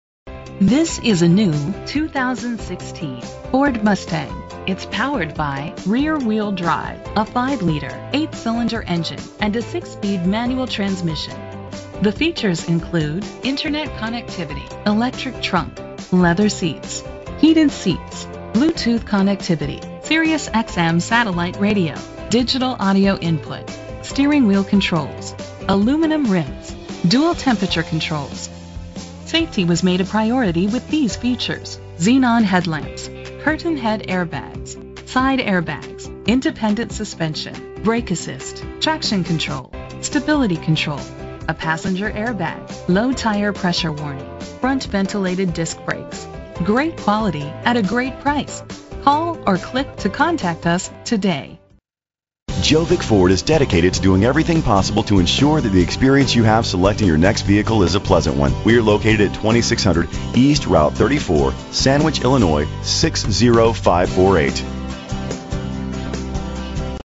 2016 Ford Mustang - Sandwich IL
Phone:
Year: 2016
Make: Ford
Model: Mustang
Trim: GT Premium
Engine: 5.0 liter 8 cylinder 32 valve
Transmission: 6-Speed Manual
Color: Shadow Black
Mileage: 5
Address:
2600 Route 34